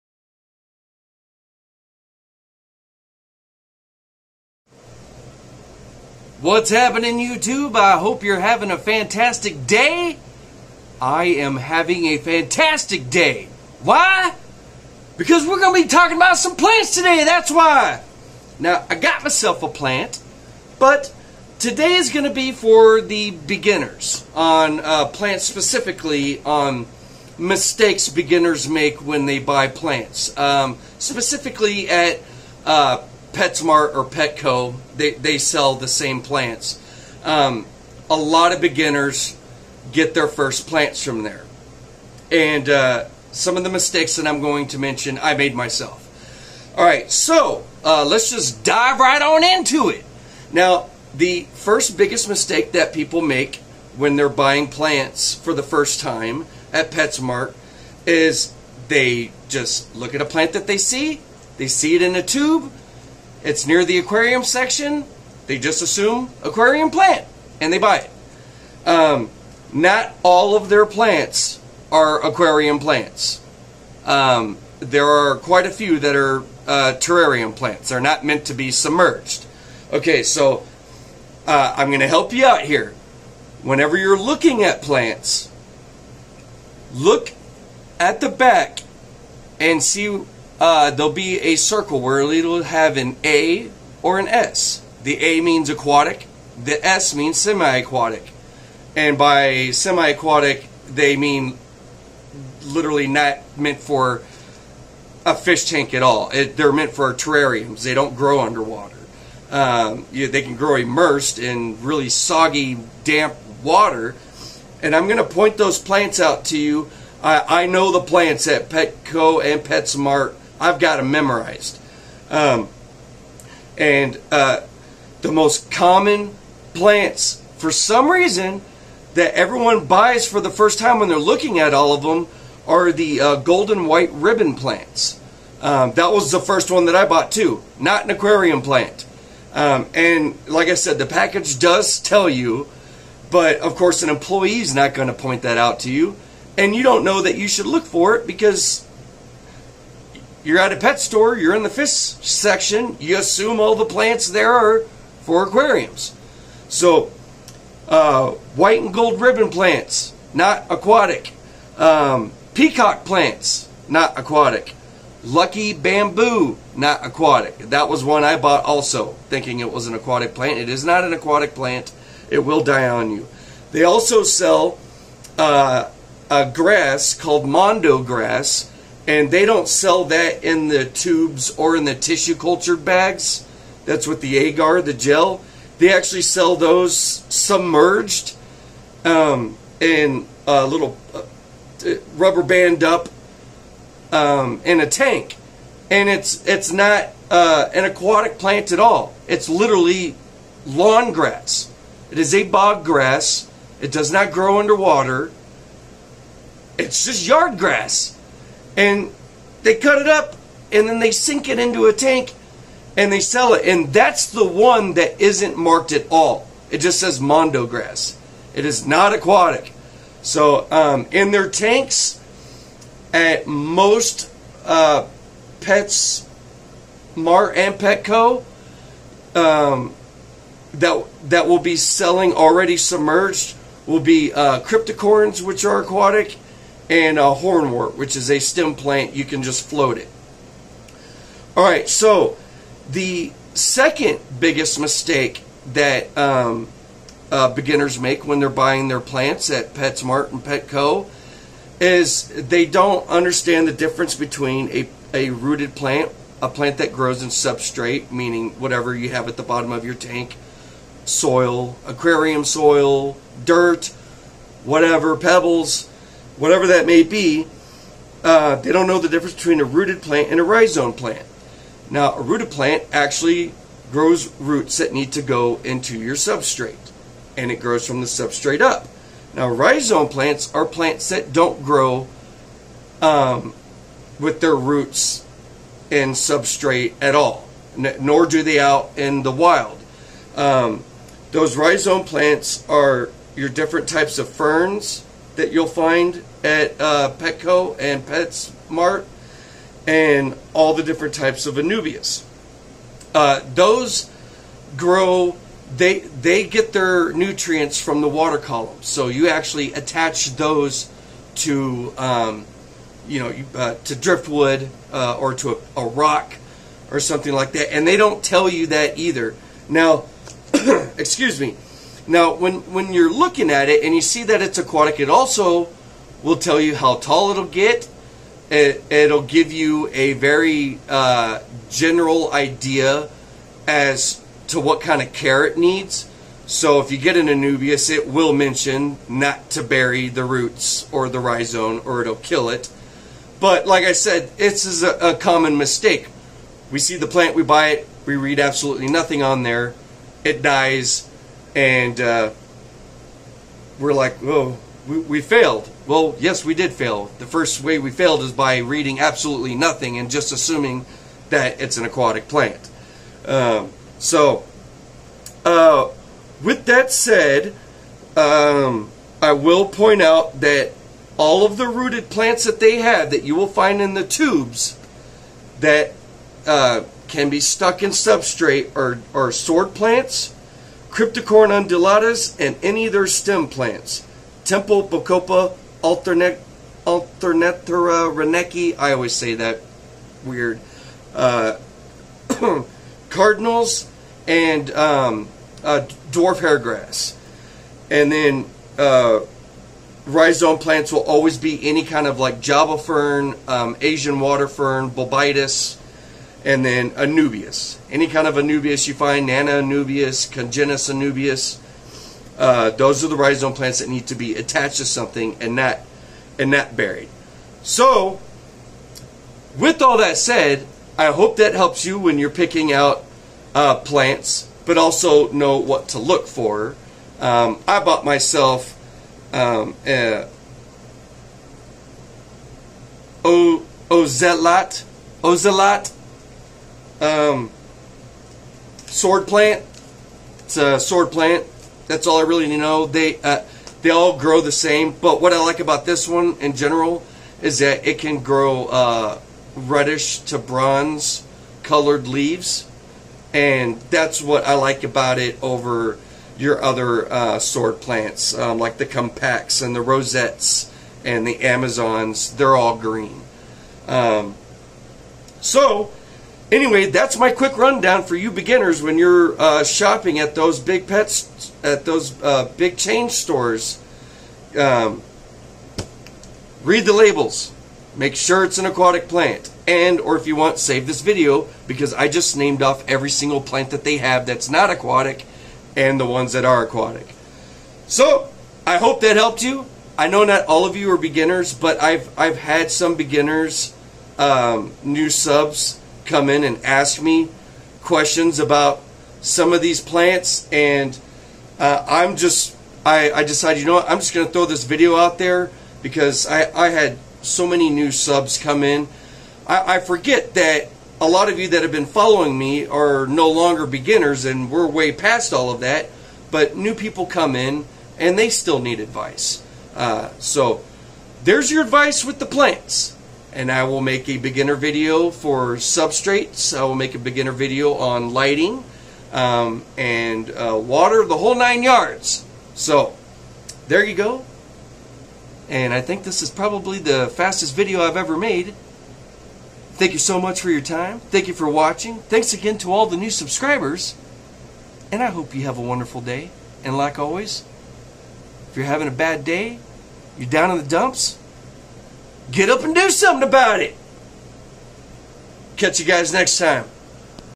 Buying, Aquarium Plants, for beginners
Many beginners, getting into the Planted, Aquarium, hobby, for the first time buy the wrong plants.
This video will help you know what to look for to make sure the plant you're buying is even meant for Aquariums!